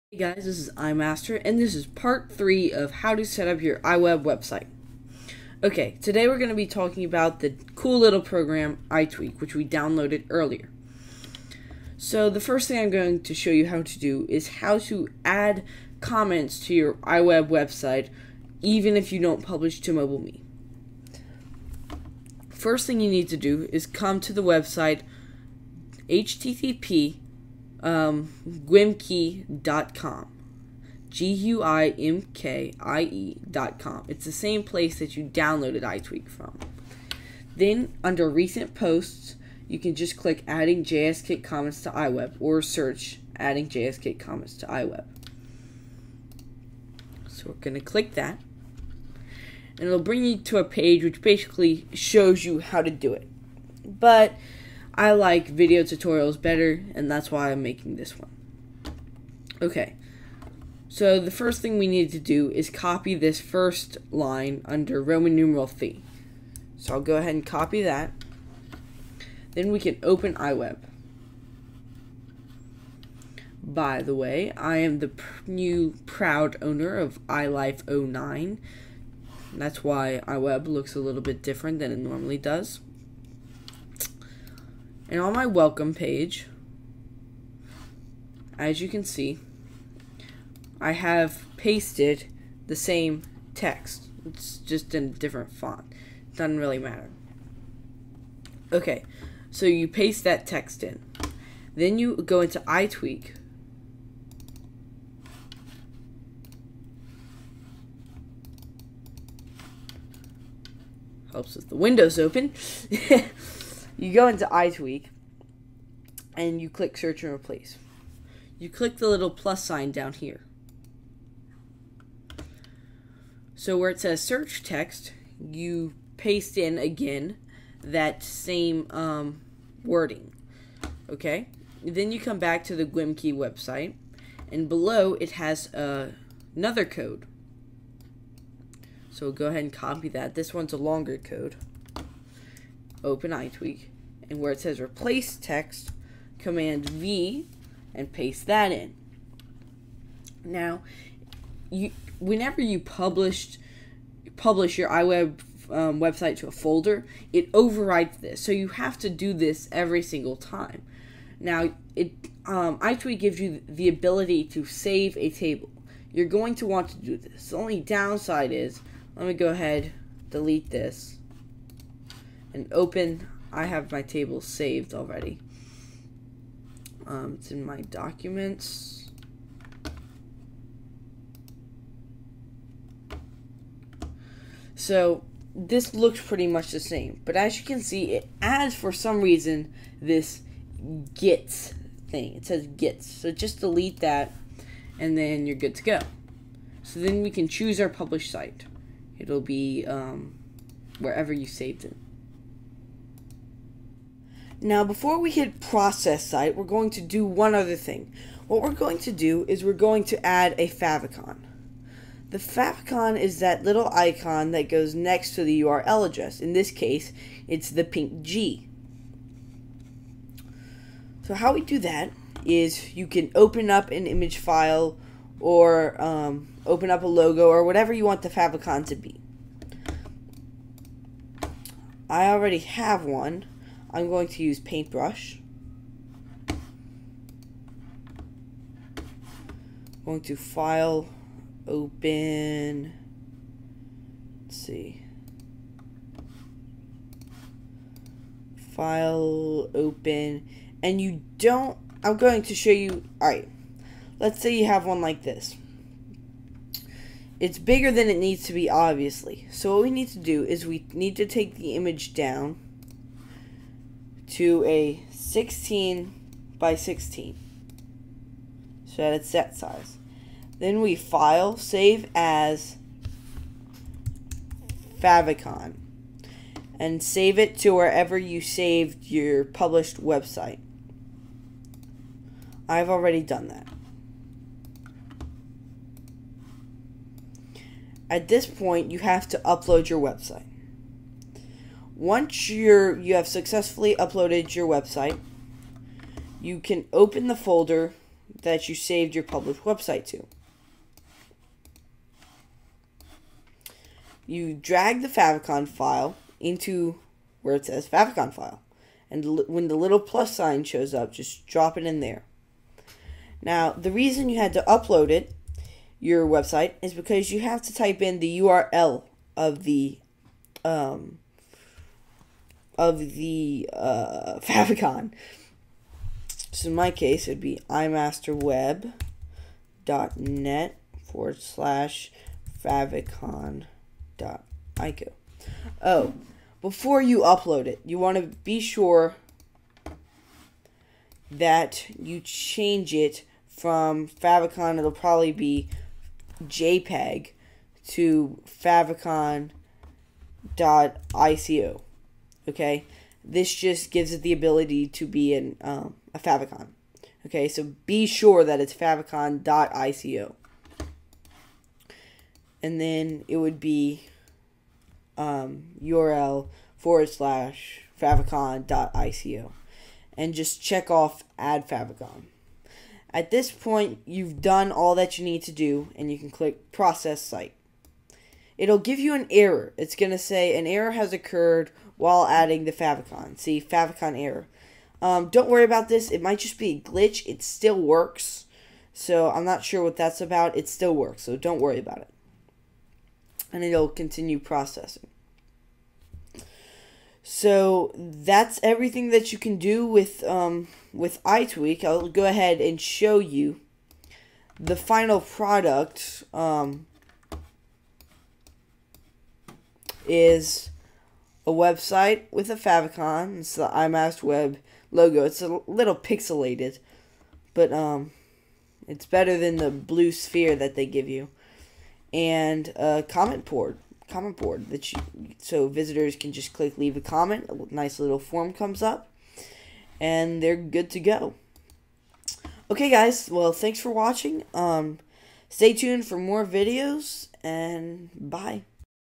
How to Set Up Your iWeb Website pt 3
Part 3 of the 3 part tutorial on how to get your iWeb website up and running. I teach you how to add a comment box to your site without MobileMe and how to add a favicon with the free program iTweak.

NOTE: when doing the Search and Replace make SURE that "search and replace codes" is checked!